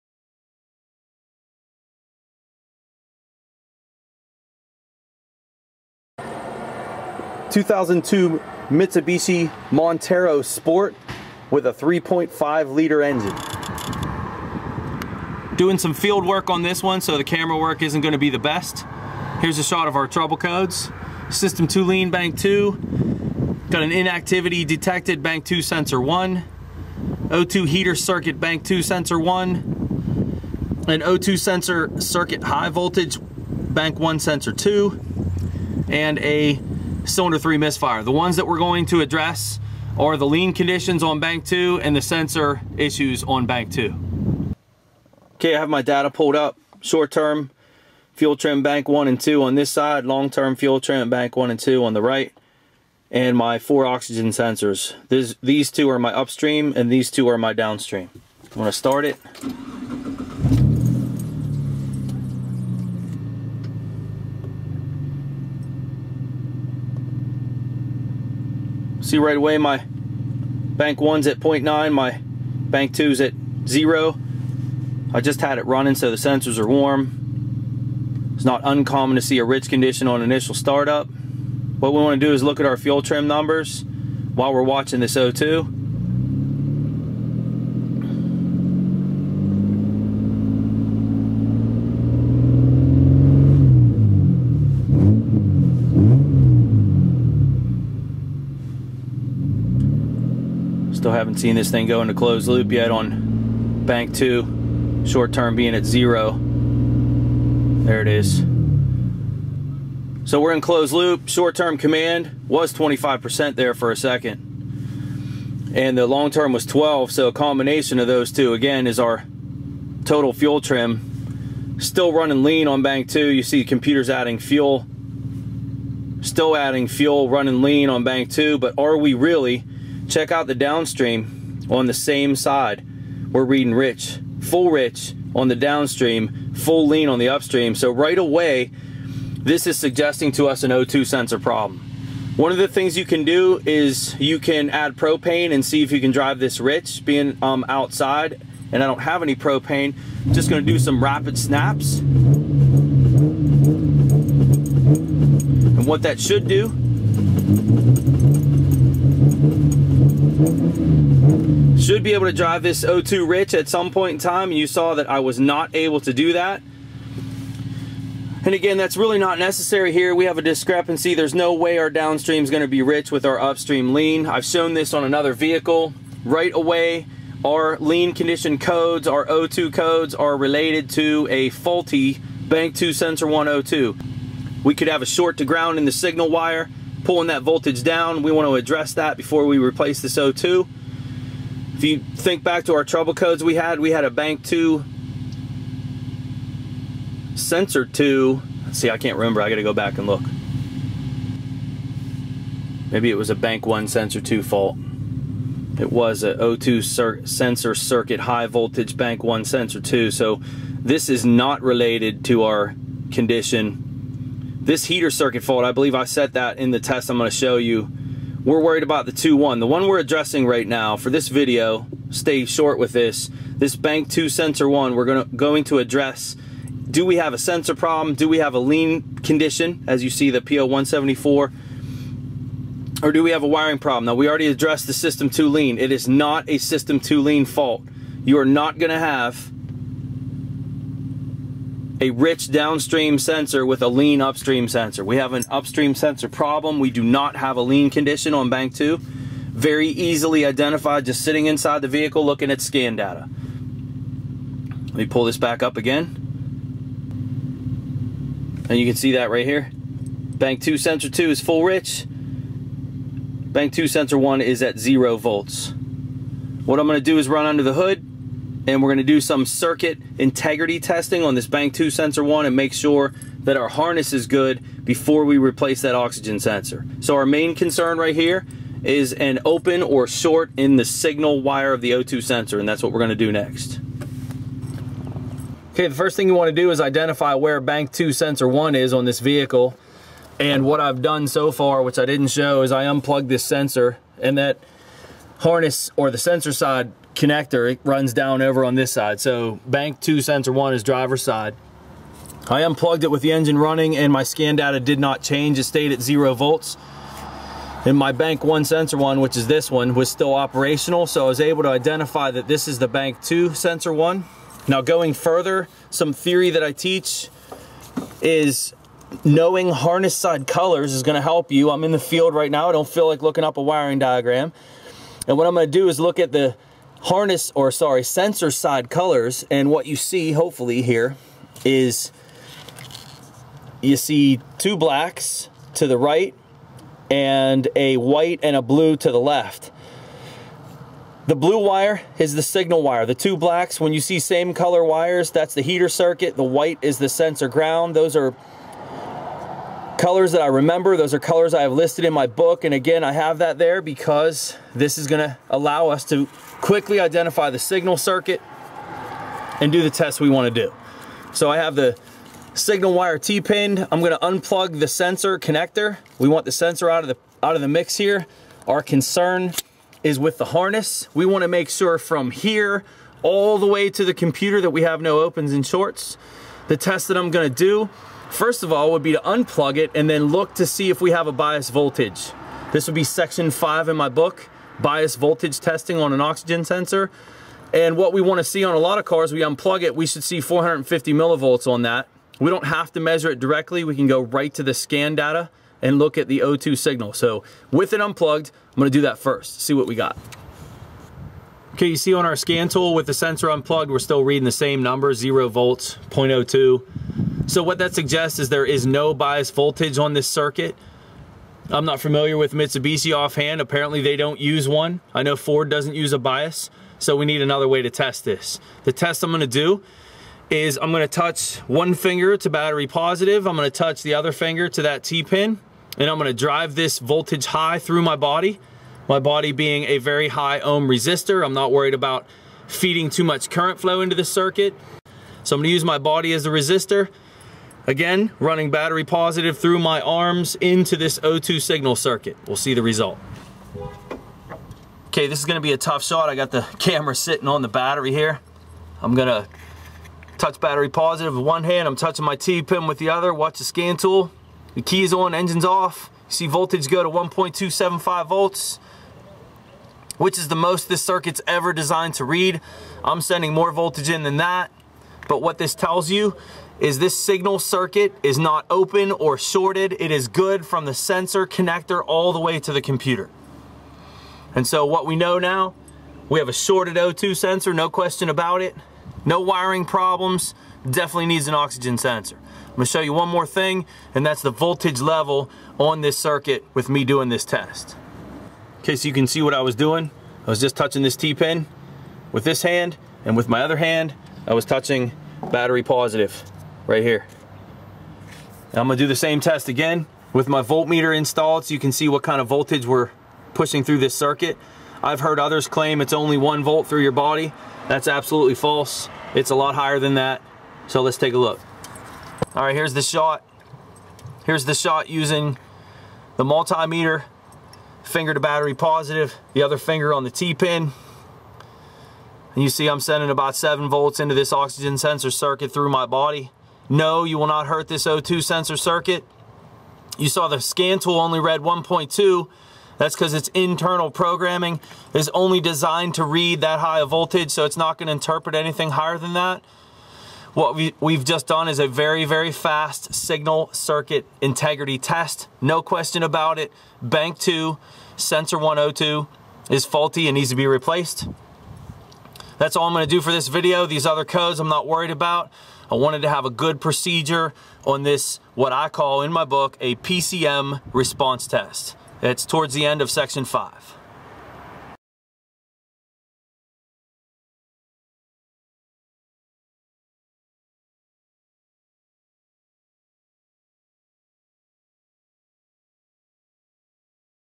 O2 sensor testing (fixed lean) P0174, P0154, P0155 - Mitsubishi
This video is a great demonstration of the quick and effective testing methods I teach for O2 sensors. No service information or wiring diagrams required here! All methods shown apply to any four-wire zirconia O2 sensor you’ll come across in the field, not just Mitsubishi.

Engine Performance Diagnostics chapter 5

Symptoms
  - check engine light (P0174, P0154, P0155, P0138, P0303)

Fix
  - replaced the bank 2 sensor 1 O2 sensor

Tests shown
  - using scan data (O2 signal and fuel trims) to evaluate O2 sensor performance
  - how to use a downstream O2 signal to check if the upstream sensor is lying
  - how to use rapid throttle snaps to create a rich condition without propane/carb clean
  - how to use scan data while unplugging sensors to identify sensor locations
  - using sensor-side O2 wire colors to identify signal and heater circuits without a diagram
  - checking for a bias voltage on a O2 sensor signal circuit
  - how to perform an O2 signal circuit integrity test
  - measuring voltage levels on an O2 signal circuit during the integrity test

Tools used
  - scan tool
  - graphing multimeter